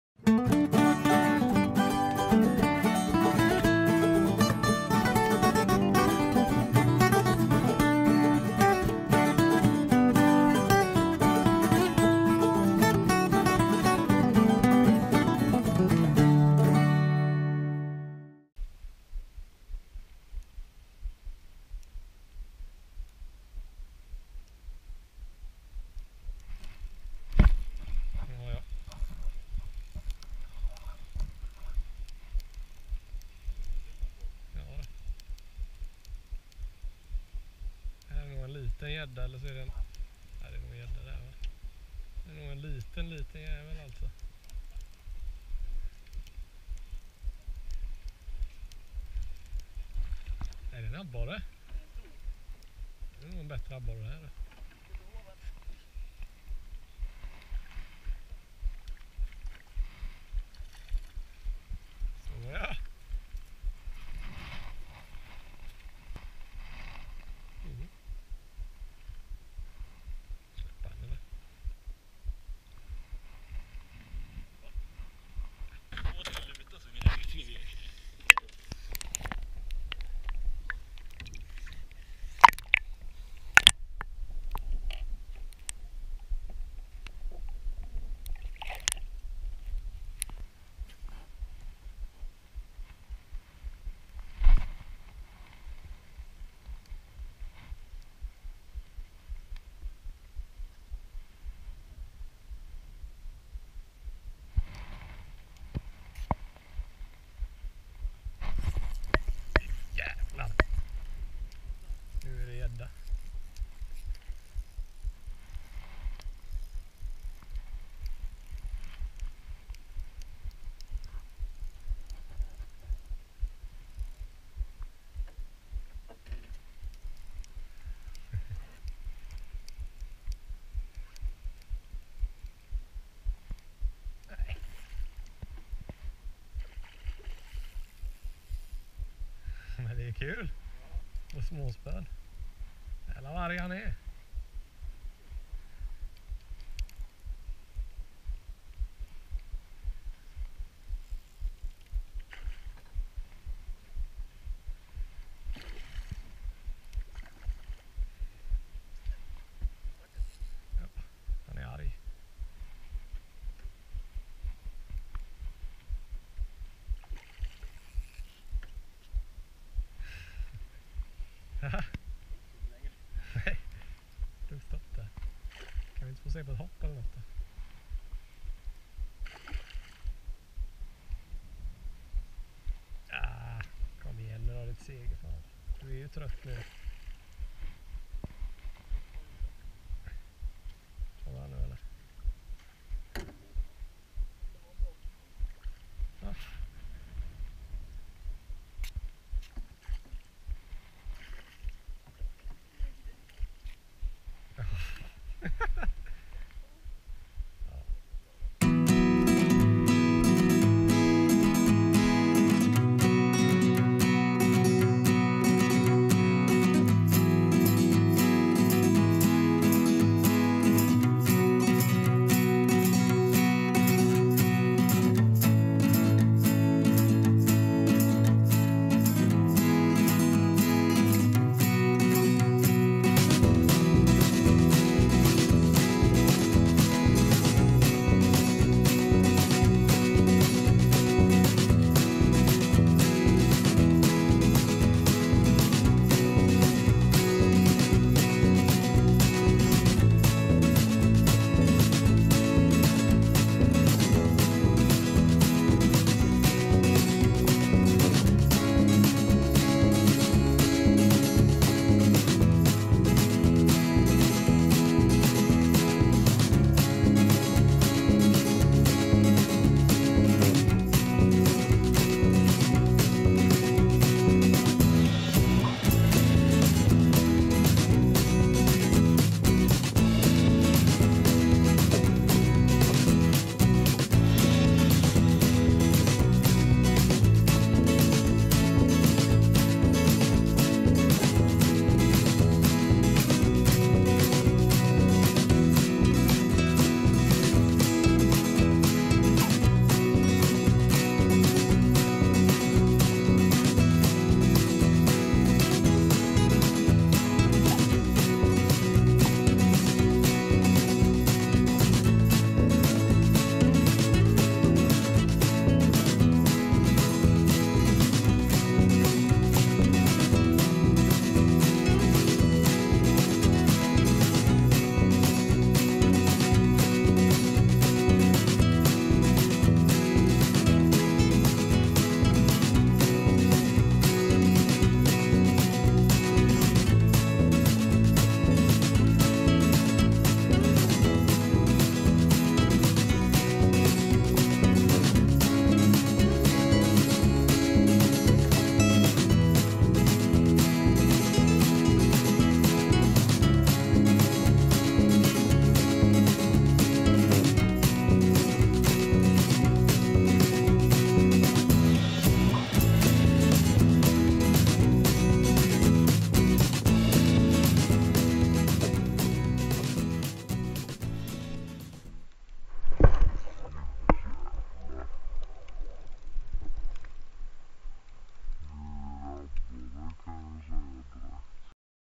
Abborrfiske - Ep Loggen #1
Nu följer vi stimmet och drar igång med lite vloggande då vi hamnat på lite utspridda hörn och inte har all tid att fiska tillsammans längre!
Här har vi vlogg nummer ett med liten trevlig kvällstur med härligt abborrfiske!

Följ oss också på facebook!